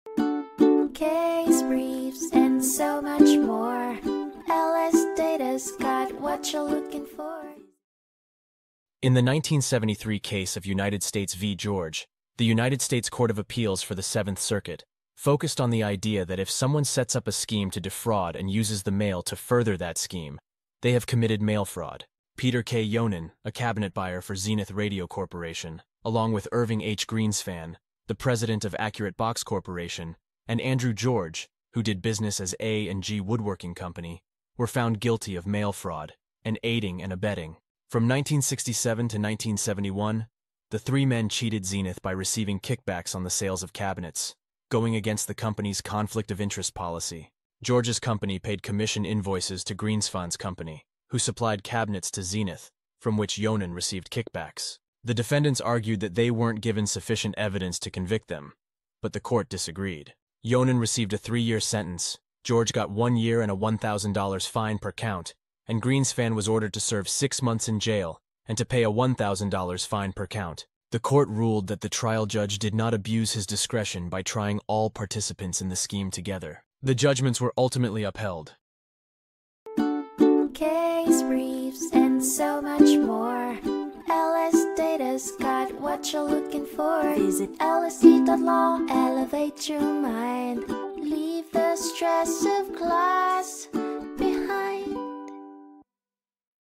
United States v. George (1973) Overview | LSData Case Brief Video Summary
Three people were found guilty of mail fraud and aiding and abetting for cheating Zenith Radio Corporation. They received kickbacks on sales of cabinets to the company, breaking Zenith's conflict-of-interest policy. Yonan found cabinet vendors and received kickbacks from Greensphan's company, which paid commission invoices to George's company. The defendants argued that the evidence wasn't sufficient to convict them, but the court disagreed.

United States v. George (1973)
477 F.2d 508